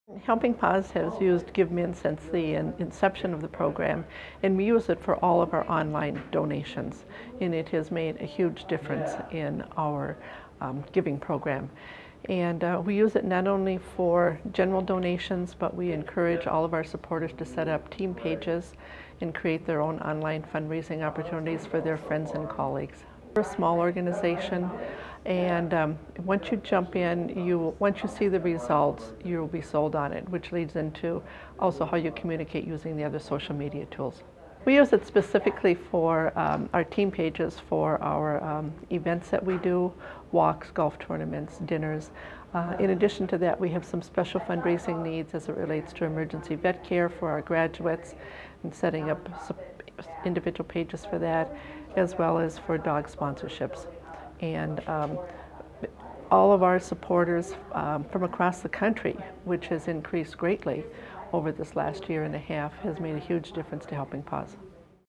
Why we use GiveMN - Pam
Helping Paws Executive Director Pam Anderson shares how her organization uses GiveMN.